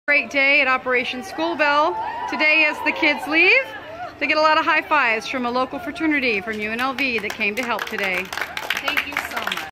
ALLV High Five for Operation School Bell!
Before the students get on the school bus with their new clothes they get cheers and high fives from UNLV students.